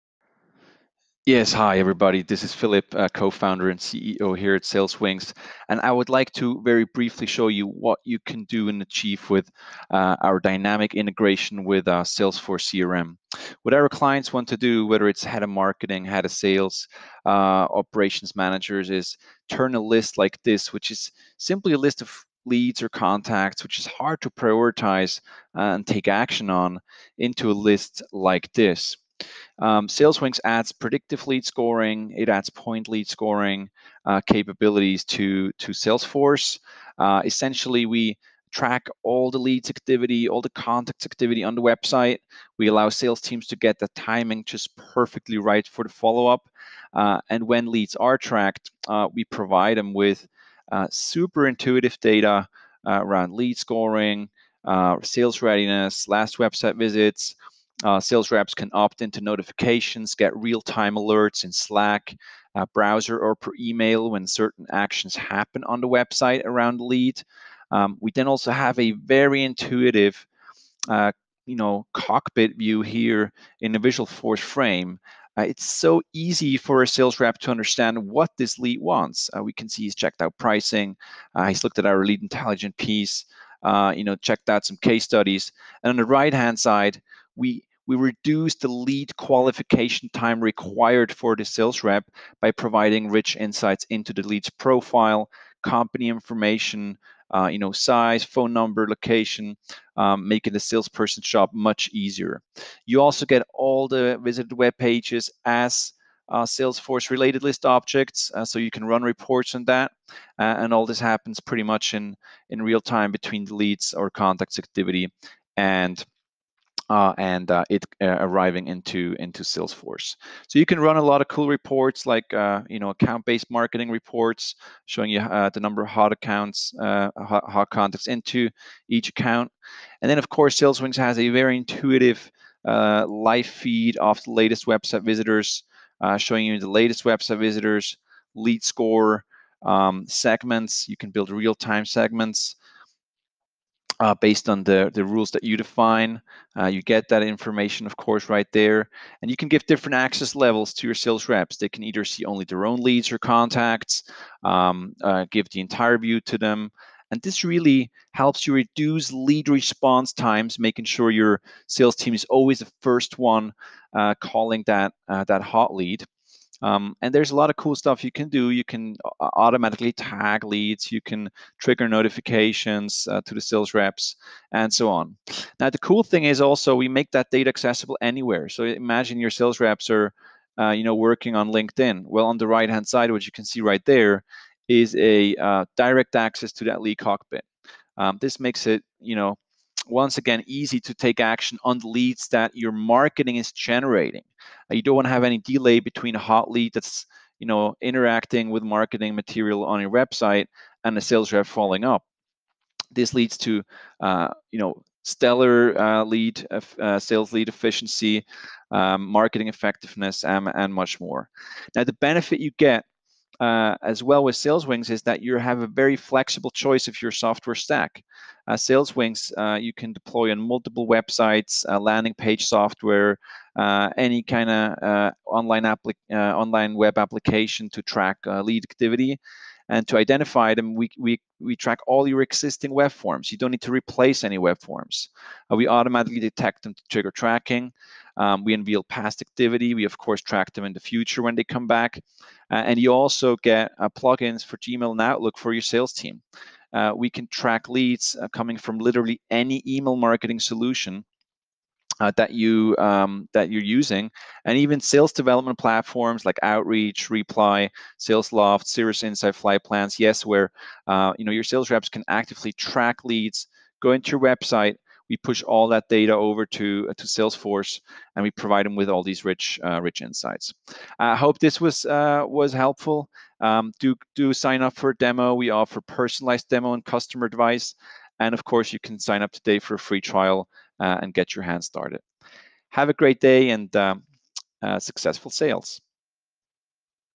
Lead Scoring and Website Tracking for Salesforce Sales Cloud
You can easily integrate website activity data and lead scoring into Salesforce generated from any email or form solution out there.